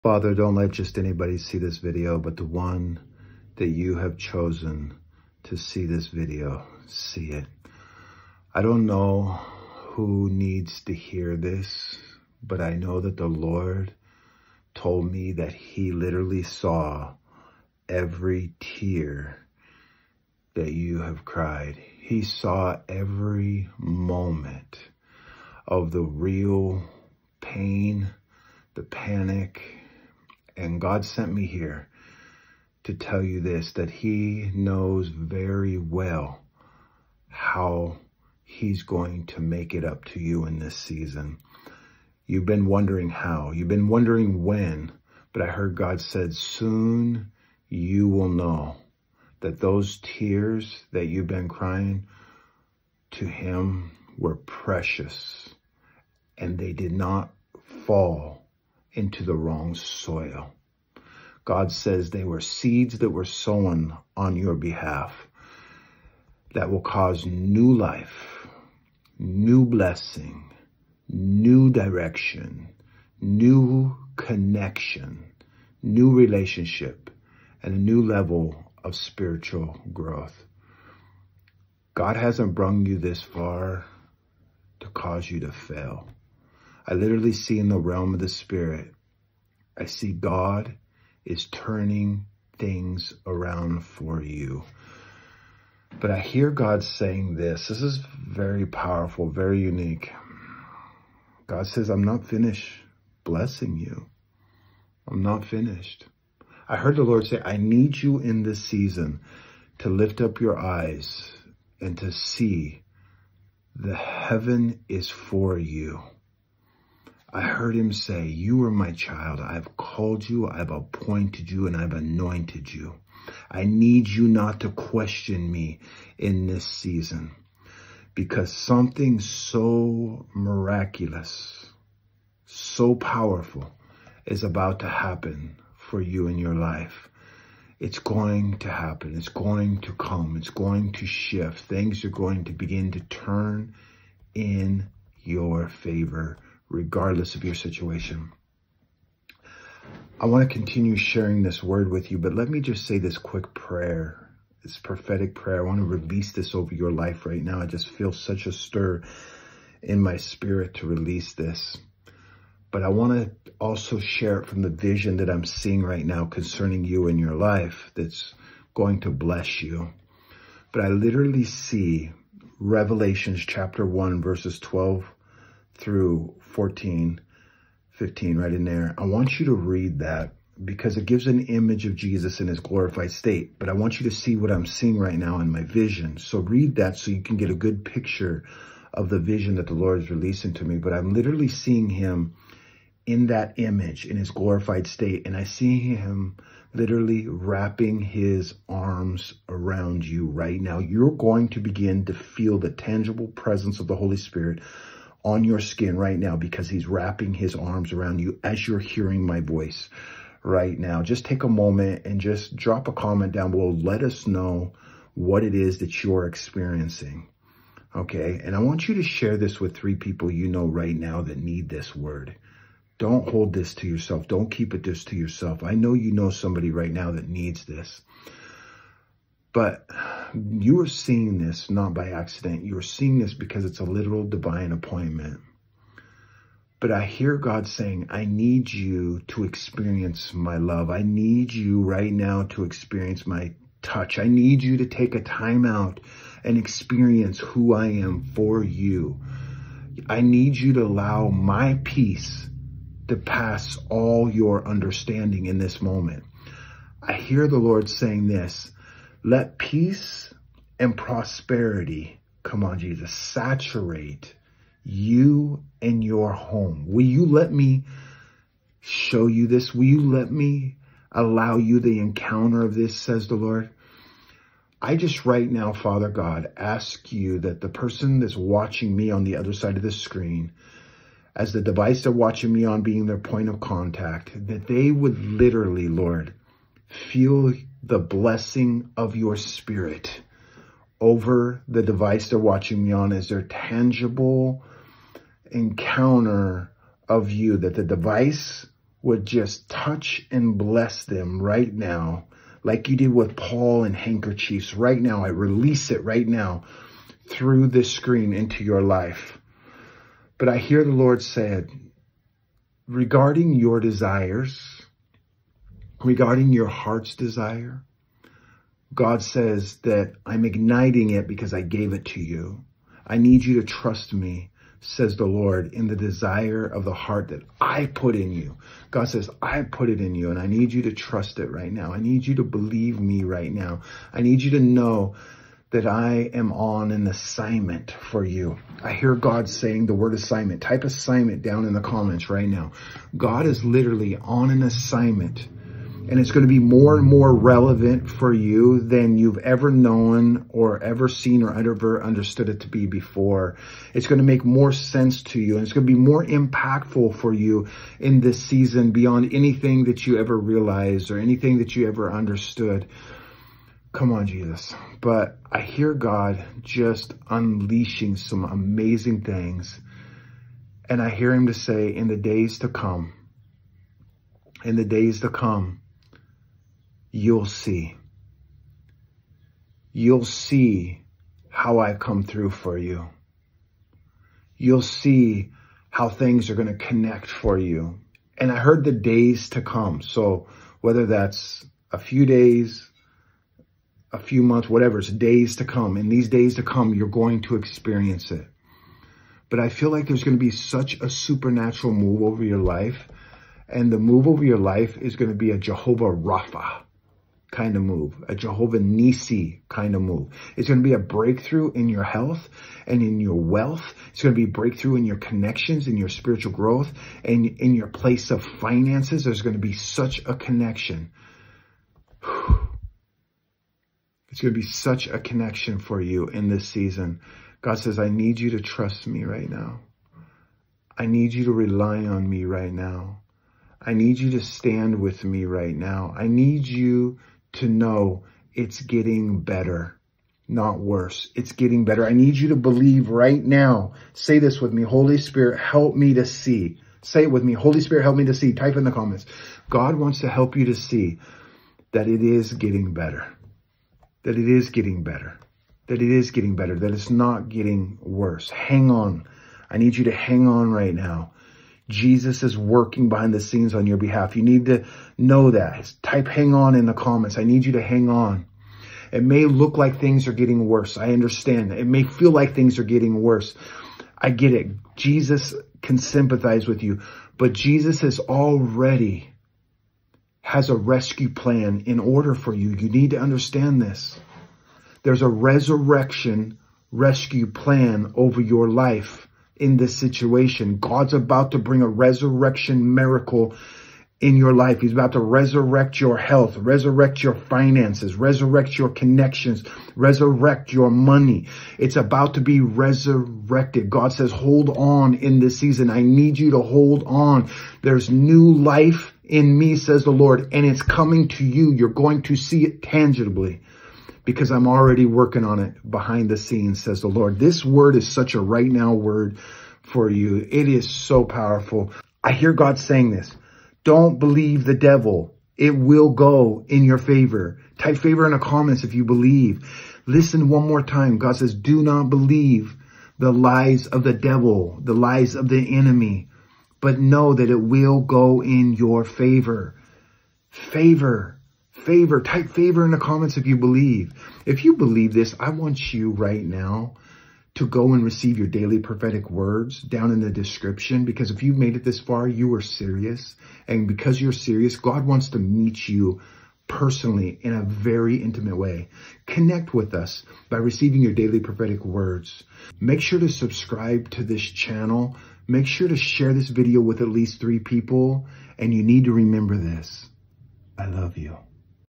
No More Delays | Prophetic Word | by PaulyB
🔥Receive Daily Prophetic Words🔥

Sow your Best Seed into the prophetic Soil of this word over your situation.

"🙏 **A Message of Hope:** The Spirit of God has a message just for you! If you've been feeling overwhelmed, crying out in pain, or experiencing delays in your life, this video is for you. God sees your tears, hears your cries, and cherishes every single one. It's time for a season of breakthrough and blessings, no more delays. Watch this powerful video to receive the message of hope and encouragement straight from the heart of God. Your time is now!